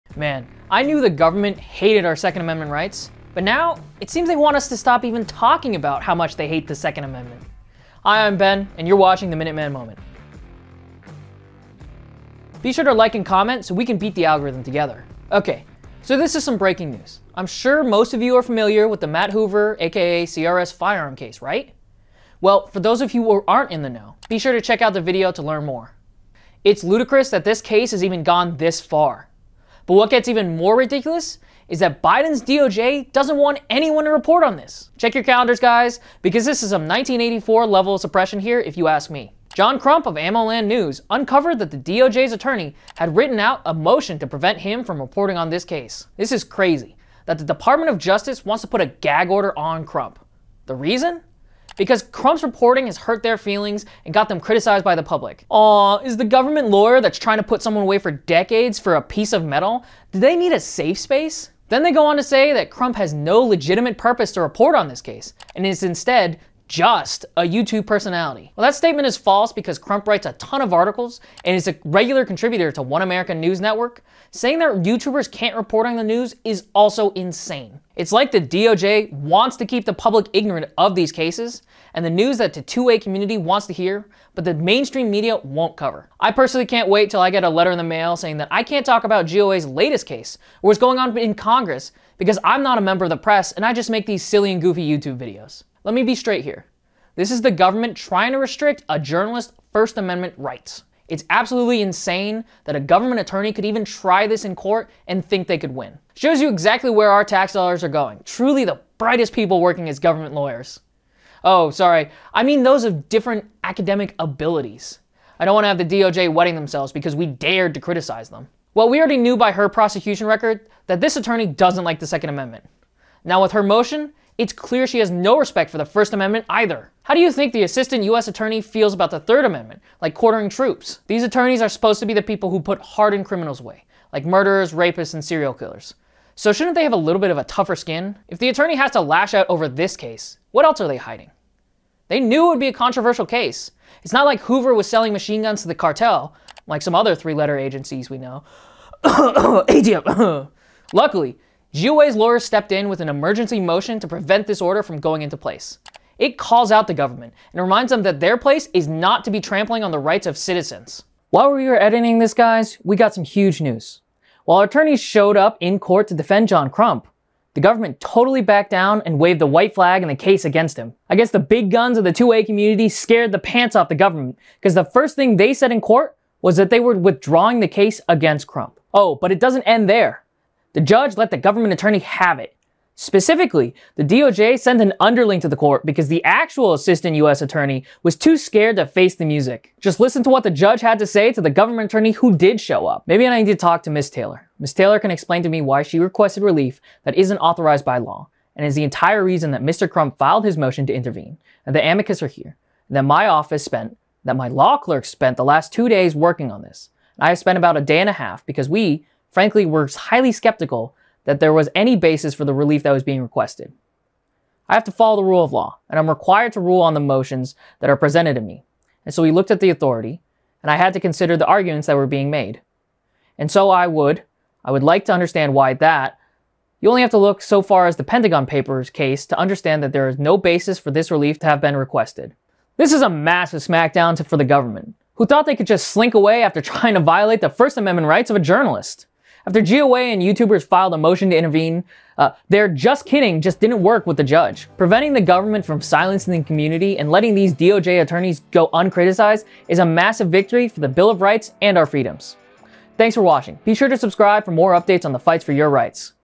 GOA Fights For The 1st AND 2nd Amendment
Today on the Minuteman Moment, Ben details the current situation between John Crump, a reporter for AmmoLand news and OAN Contributor, and his coverage of the CRS Firearms case being placed under a gag order by an overzealous federal prosecutor.

Luckily, GOA was able to step in with our lawyers and appeal this wild infringement of a journalist's first amendment rights, resulting in the DOJ dropping the order.